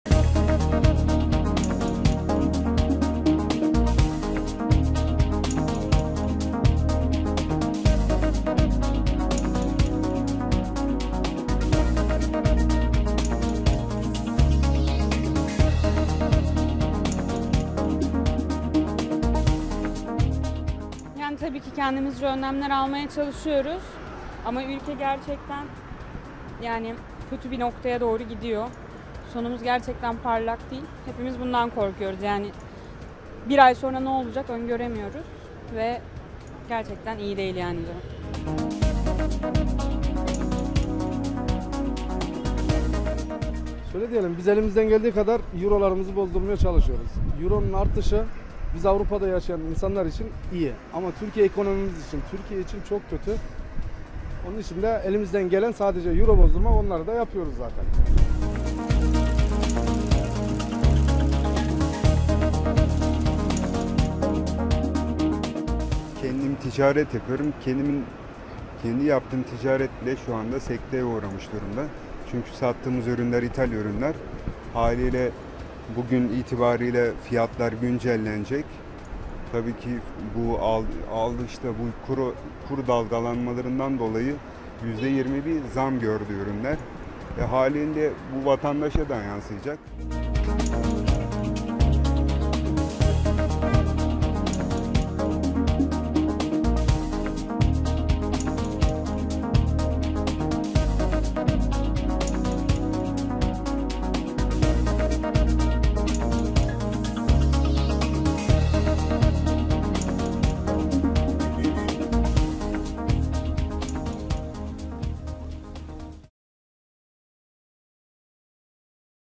Ekonomik kriz hakkında vatandaş ne düşünüyor? - DW Türkçe
Türkiye tarihinin en büyük ekonomik krizlerinden birini yaşıyor. Merkez Bankası aldığı çeşitli önlemleri açıklarken, Maliye Bakanı Berat Albayrak'tan da "aksiyon planının" devreye sokulduğu açıklaması geldi. Peki kriz hakkında vatandaşlar ne düşünüyor?